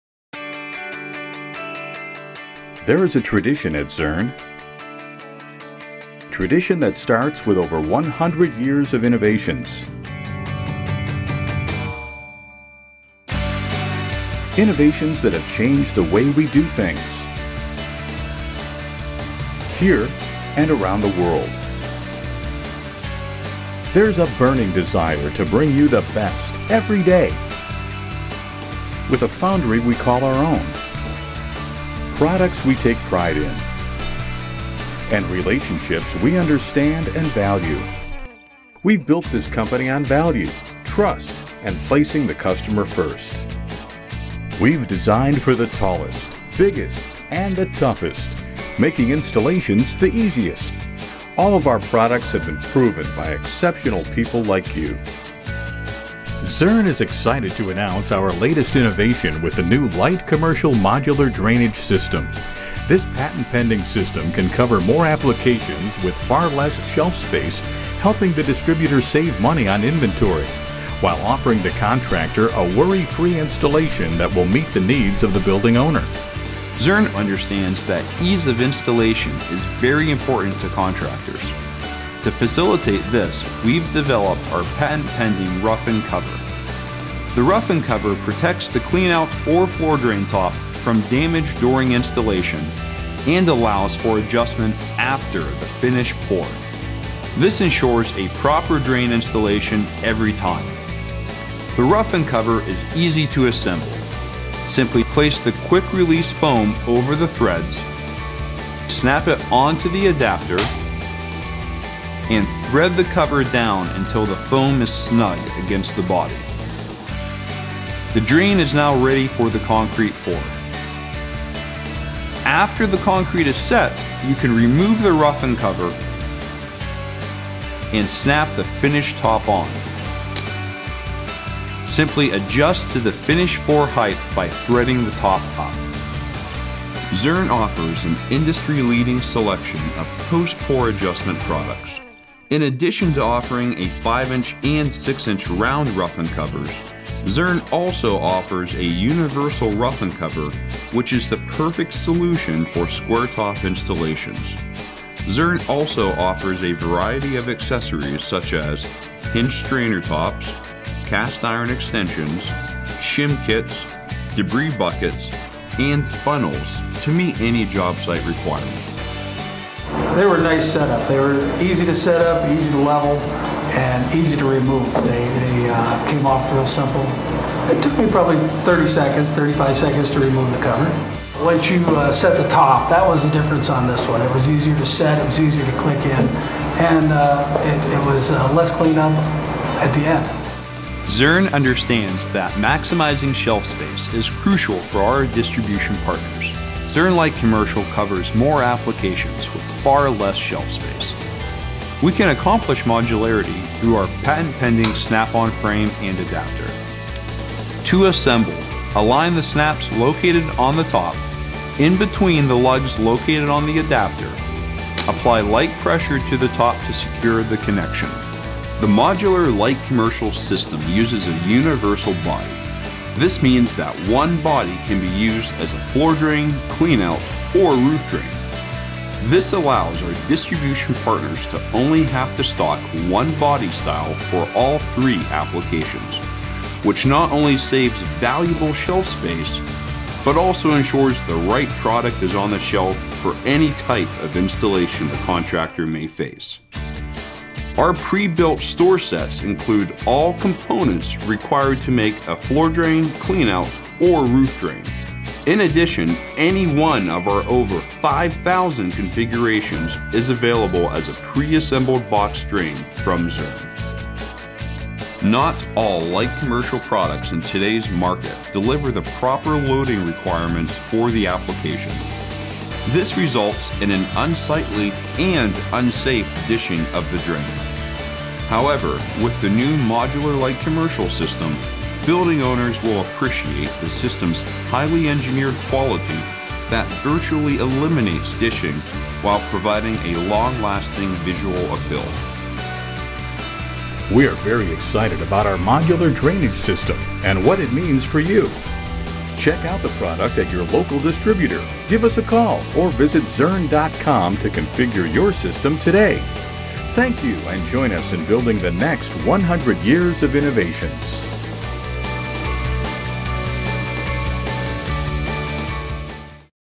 Zurn Floor Drains Light Commercial Modular Drainage System
Zurn Industries introduces the new patent-pending line of commercial modular drainage products.  This innovative product line is an industry-exclusive, offering unparalleled flexibility for contractors, distributors, and building owners.